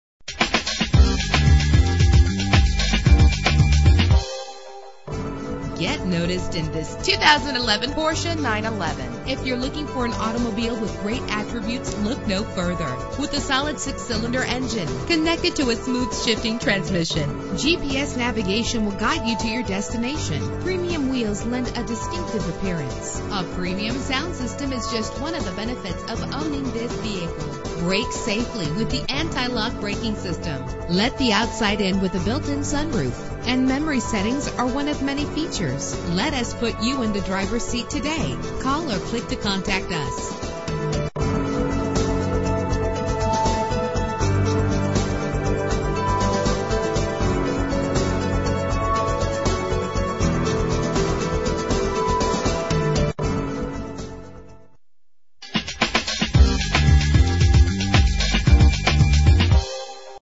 2011 Porsche 911 - Riverside CA
Year: 2011
Make: Porsche
Model: 911
Trim: Turbo
Engine: 3.8 liter H6 24 valve
Transmission:
Color: Carrera White
Mileage: 5
Address:
3213 Adams Street